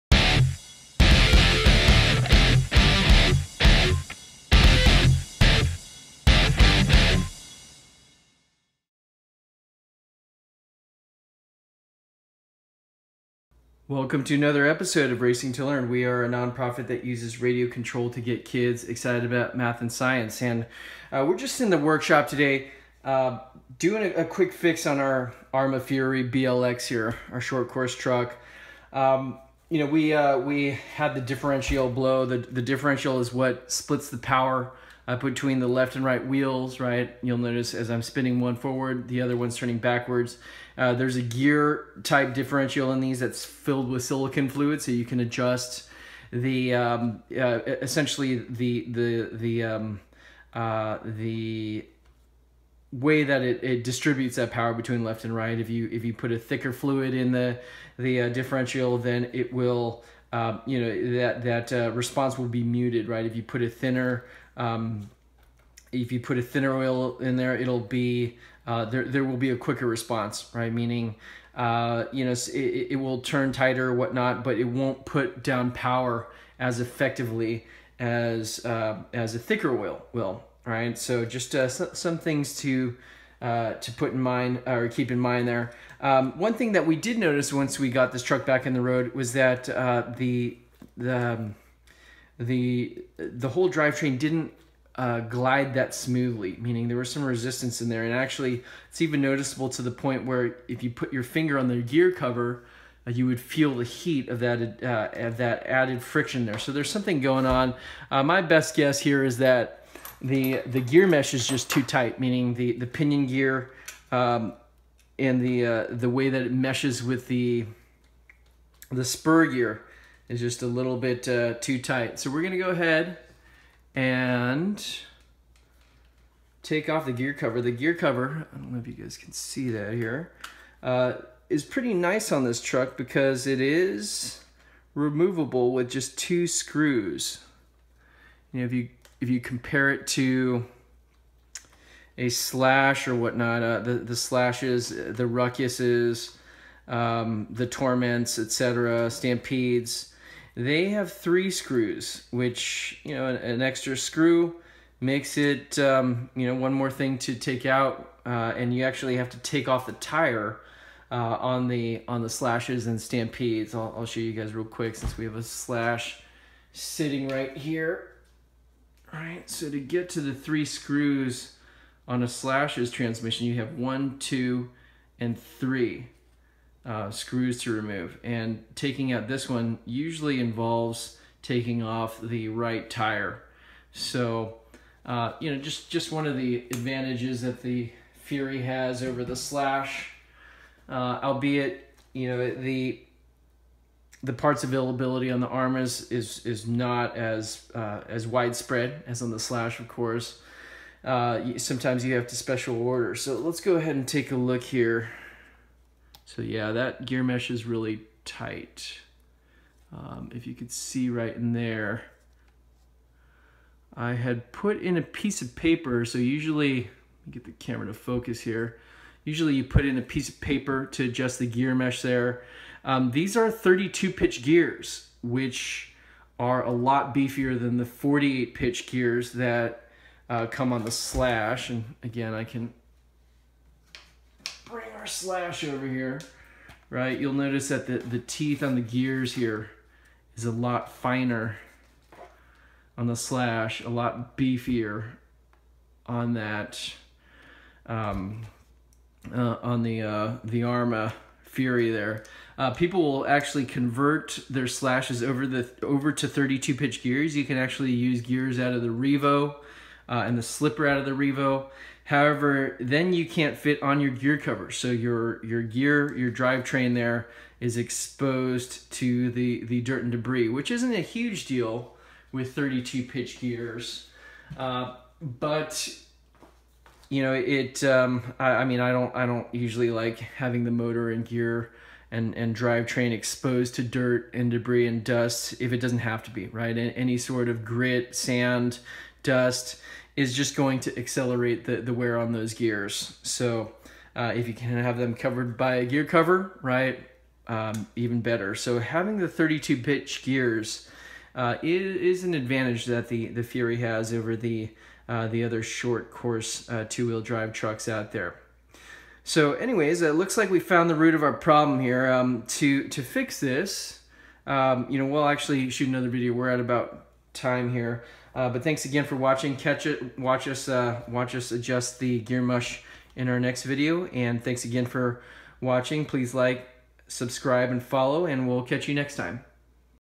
Racing2Learn: Diagnosing Arrma Fury BLX Gearbox, Differential, Gears, Comparison to Traxxas Slash
In a previous episode, we went through a detailed breakdown of the rear end, transmission, and differential on our Arrma Fury BLX 2WD Short Course Truck In this episode, we diagnose and discuss gearboxes, differentials, and compare the Fury to our Traxxas Slash 2WD brushless truck. The Fury BLX has 32-pitch gears, which are a distinct durability advantage over the Slash's finer toothed 48-pitch gears. Also, the Fury has only two screws that hold on the gearbox cover, which means that the tires don't have to be removed to access the gears, unlike the three screws holding the gearbox cover on a Slash/Stampede/Rustler.

More videos on the Fury, and also the Traxxas Slash and ECX Ruckus that we've evaluated for our platform: